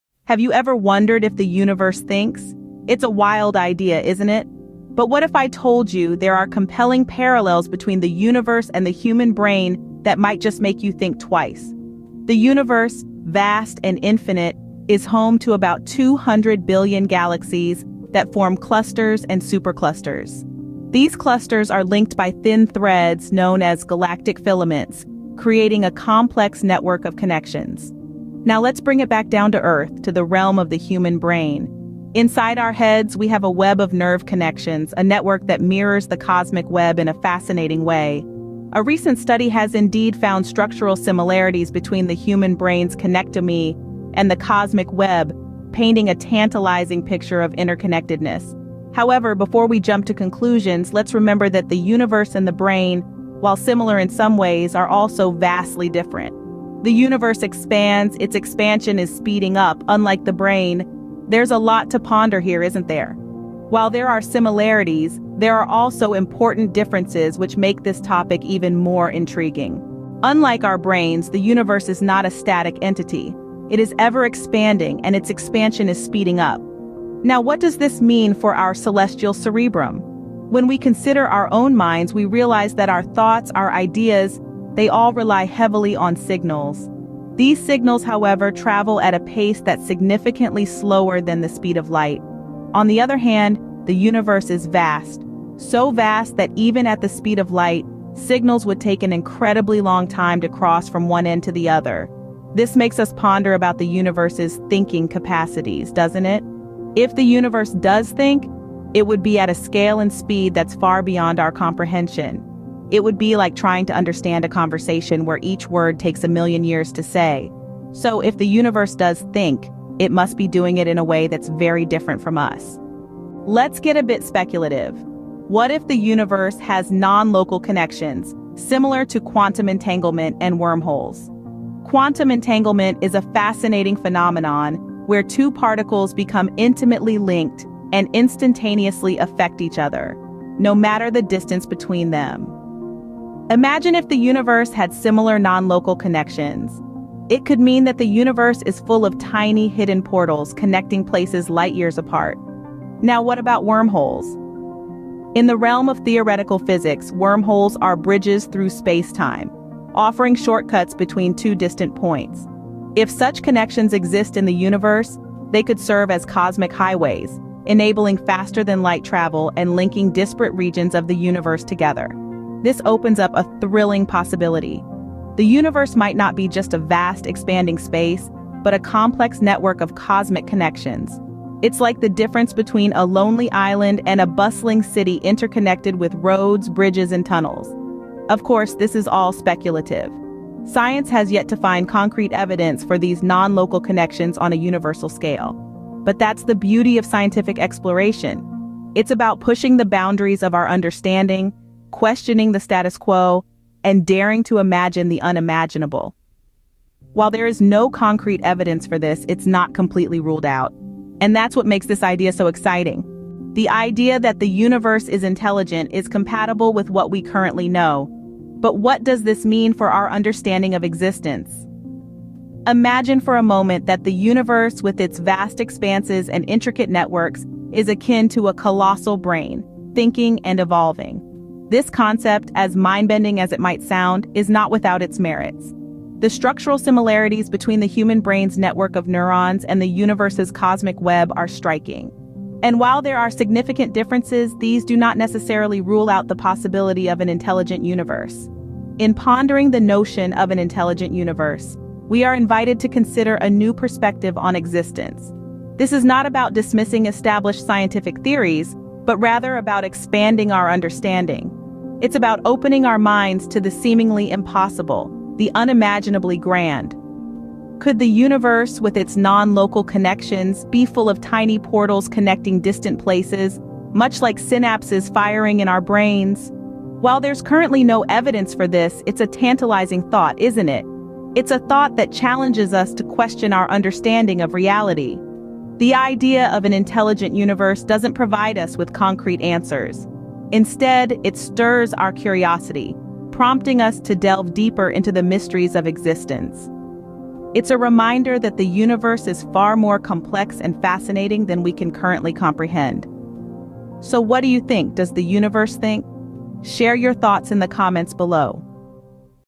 Maybe the Universe Thinks. Hear Me Out
Visuals from pixaby.com,  pexels.com,   vecteezy.com The article "Maybe the Universe Thinks. Hear Me Out" discusses the possibility that the universe might possess thinking capabilities, similar to how the human brain functions. Some key points from the article include:
The universe is structurally similar to the brain, with high connectivity and rapid signaling, which are conducive to thinking.
However, the universe is much larger than the human brain and is expanding, making it difficult for it to "think" in the same way.
There is currently no evidence for non-local connections or that they would allow the universe to think, but the possibility cannot be ruled out.
The idea that the universe is intelligent is compatible with all we know so far, even though it may sound far-fetched.
Other possibilities include the Big Bang repeating and copies of individuals living in parallel worlds, although these ideas are not supported by evidence and might never be.
In summary, while there is no concrete evidence to support the claim that the universe thinks, the idea is not contradicted by current knowledge. The article suggests that these theoretical possibilities are worth exploring and considering, even if they remain unproven.